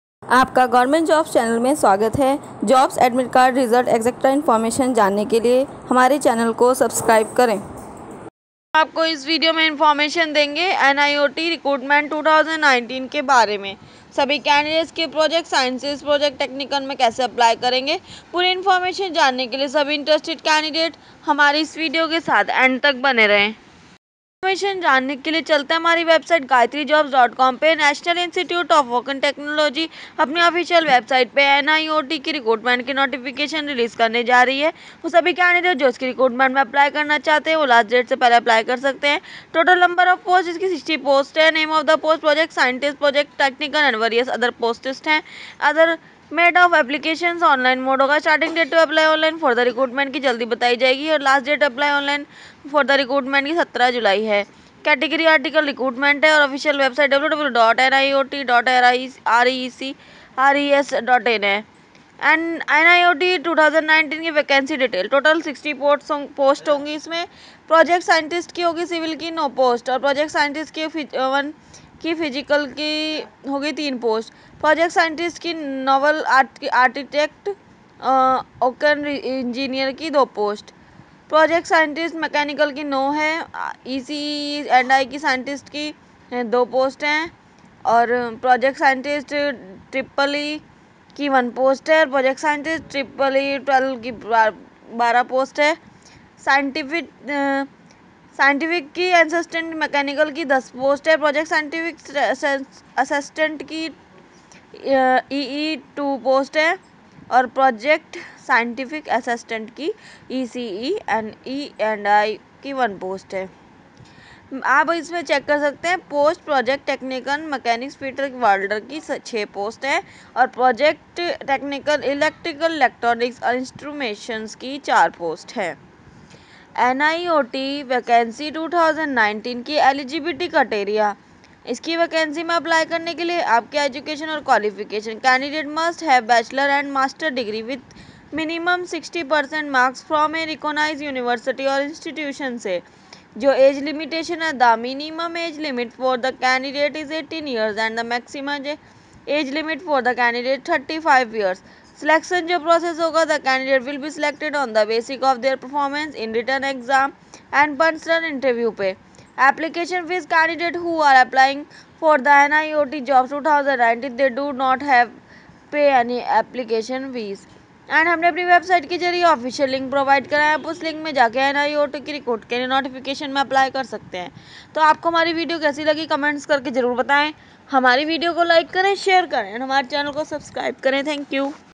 NIOT Recruitment 2019 Apply for 60 60 Project Scientist, Project Technicians
National Institute of Ocean Technology has released the NIOT Recruitment 2019 for fill up 60 Project Scientist, Project Technician and various other Posts. Apply Online NIOT Jobs by 17th July 2019 @